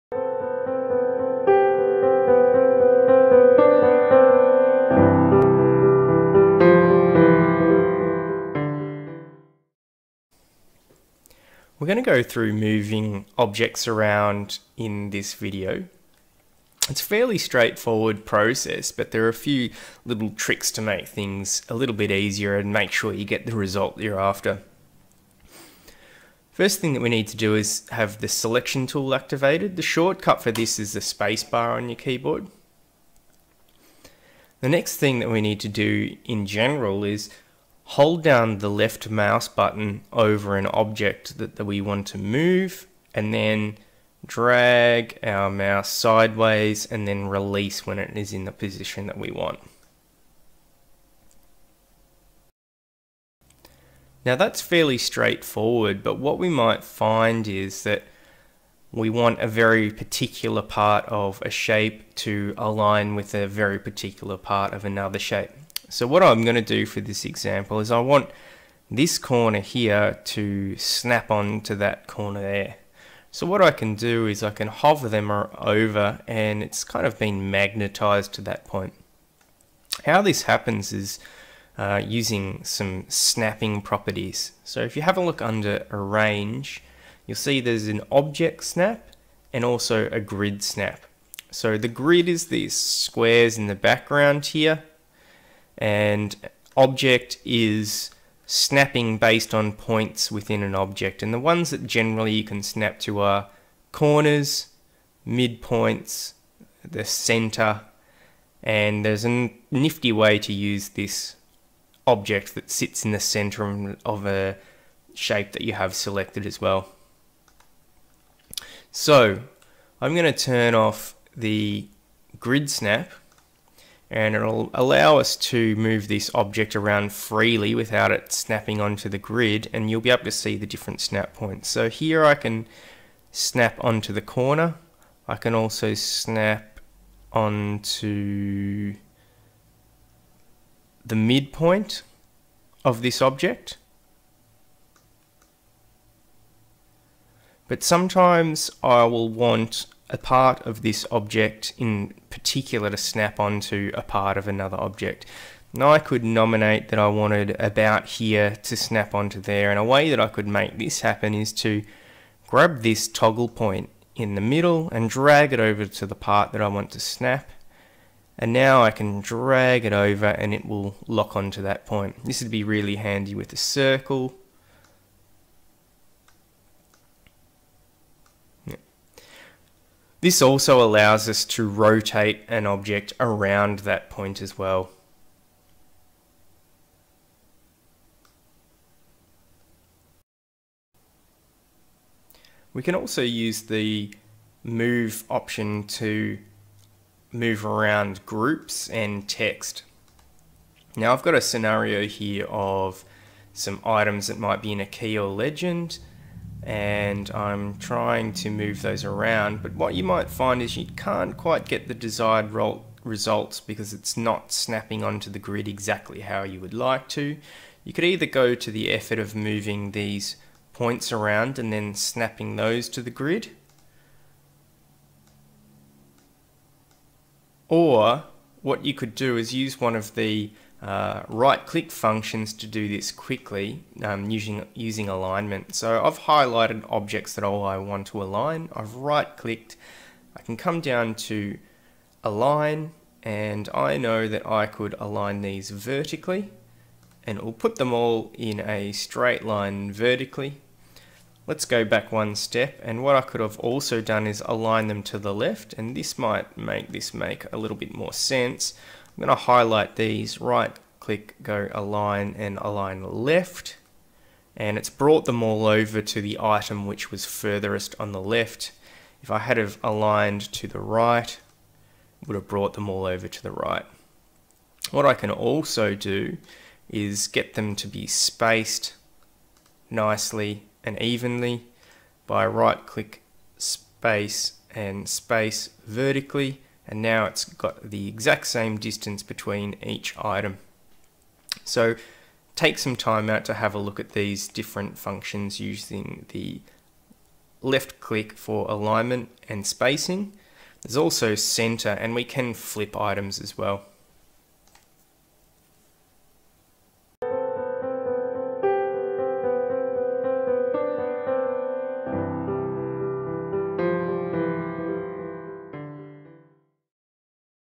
Layout for SketchUp - Moving Objects
In this video we look at moving objects in LayOut for SketchUp